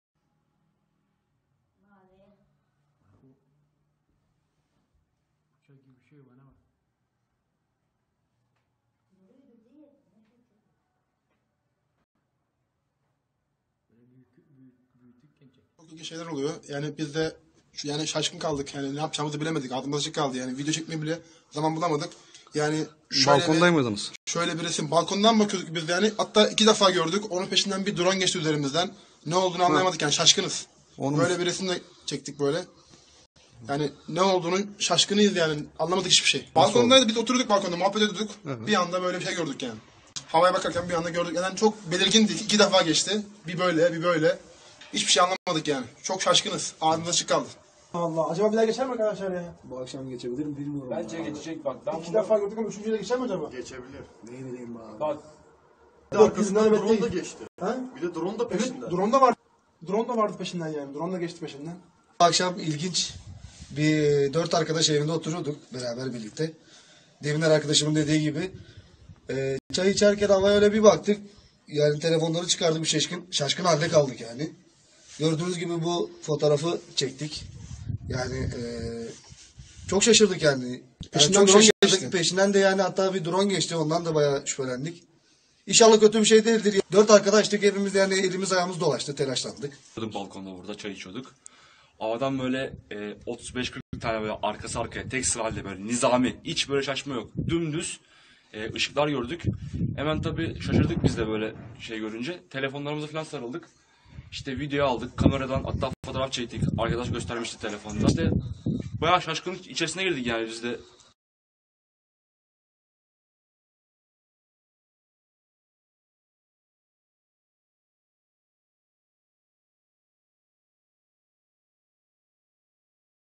GÖKYÜZÜNDEKİ ESRARENGİZ IŞIK HERKESİ ŞAŞIRTTI!
📽️ Kırklareli ve Tekirdağ'da gökyüzünde görülen esrarengiz sıralı ışıklar vatandaşlar tarafından şaşkınlıkla karşılandı.

Kırklareli'nin Lüleburgaz ve Tekirdağ'ın Malkara ilçesinde gökyüzünde görülen çok sayıda sıralı ışık vatandaşlar tarafından şaşkınlıkla izlendi. Evinin balkonunda yıldızları izlediği sırada çok sayıda sıralı ışığı gören Yasin Demiröz isimli vatandaş, o anları hayretler içinde telefonuyla görüntüledi. Kırklareli'nin Lüleburgaz ilçesinde de bir vatandaş, benzer görüntülere rastladığını ifade etti. Öte yandan gökyüzündeki sıralı cisimlerin, Starlink uyduları olduğu şeklinde yorumlar yapıldı.